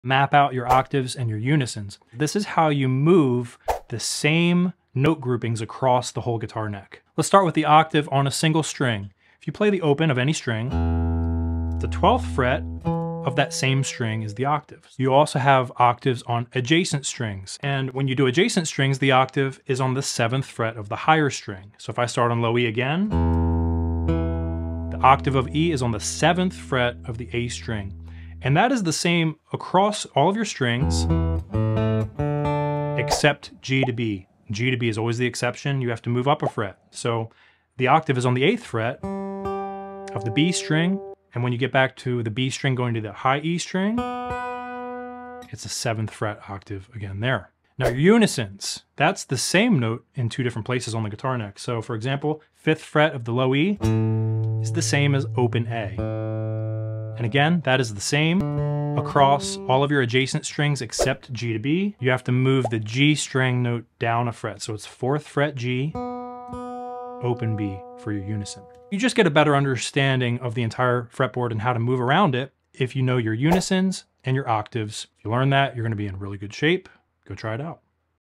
Octaves and Unisons
Use these INTERVALS to navigate the fretboard. Go try it out!